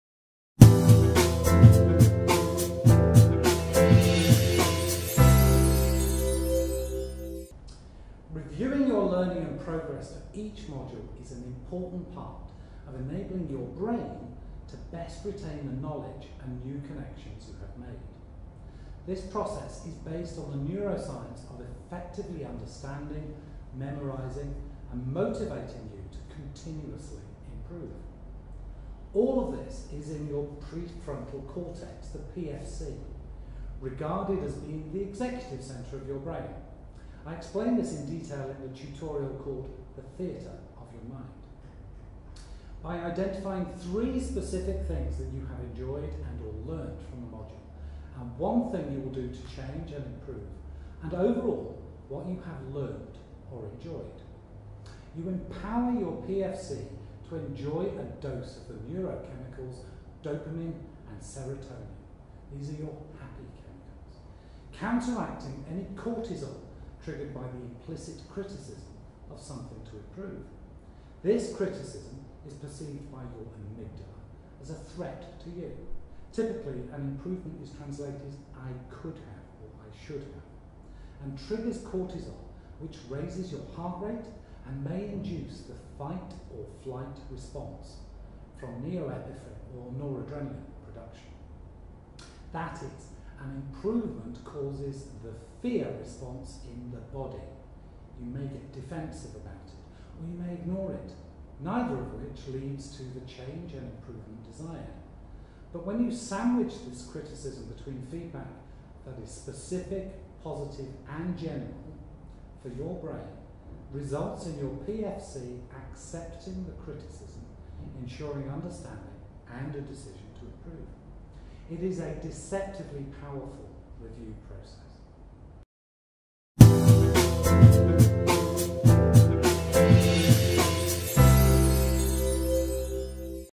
Forging Success Review
Reviewing your learning and progress for each module is an important part of enabling your brain to best retain the knowledge and new connections you have made.
This process is based on the neuroscience of effectively understanding, memorizing and motivating you to continuously improve.

All of this is using your Pre Frontal Cortex (the PFC) -- regarded as being the executive centre of your brain. I explain this in detail in the tutorial called the "Theatre of Your Mind".

By identifying 3 specific things that you have enjoyed and/or learned from the module, and one thing you will do to change and improve and overall what you have learned or enjoyed, you empower your PFC to enjoy a does of the neurochemicals dopamine and serotonin (your "happy" chemicals") counteracting any cortisol triggered by the implicit criticism of something to improve. The criticism is perceived by your amygdala as a threat to you (typically an improvement is translated as "I should have. ") and triggers cortisol which raises your heart rate, and may induce the fight or flight response from neo-epiphrine (noradrenaline) production. That is, an improvement causes the 'fear' response in the body, you may get defensive about it, or you may ignore it... neither of which leads to the change and improvement desired. Sandwiching this criticism feedback between specific positive and general positive feedback for your brain results in your PFC accepting the criticism, ensuring understanding and a decision to improve.

It is a deceptively powerful review process.